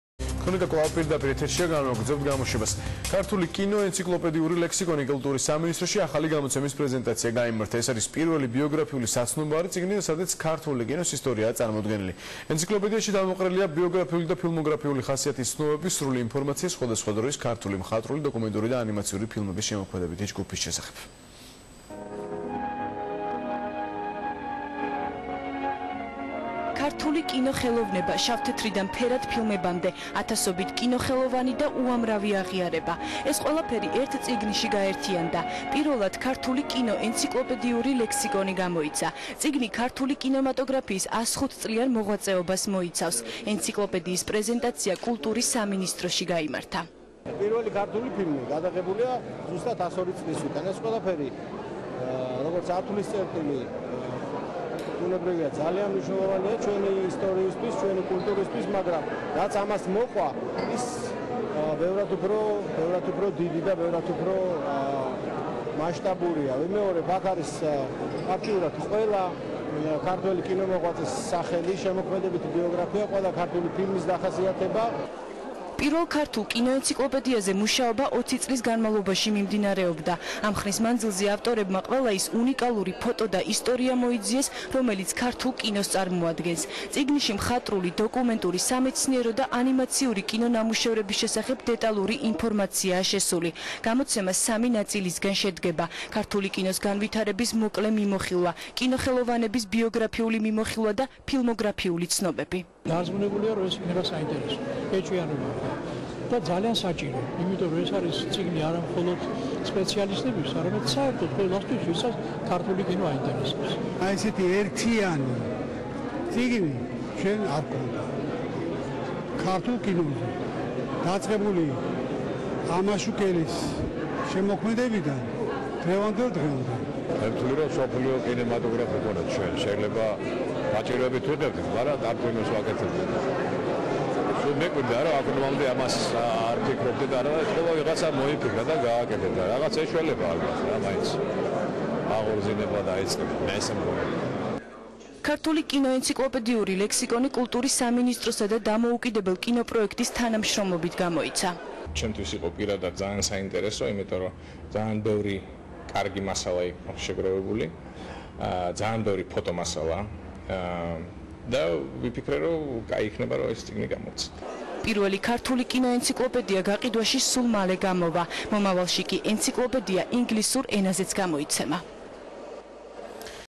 ქართული კინოენციკლოპედია
საქართველოს კულტურისა და ძეგლთა დაცვის სამინისტროში გაიმართა „ქართული კინო. ენციკლოპედიური ლექსიკონის" პრეზენტაცია.
კომპანია „დამოუკიდებელი კინოპროექტის" პროექტი „ქართული კინო. ენციკლოპედიური ლექსიკონი" პირველი ბიოგრაფიული საცნობარო გამოცემაა, რომელშიც სახელებით წარმოდგენილია ქართული კინოს ისტორია საქართველოში მისი შემოსვლიდან დღემდე.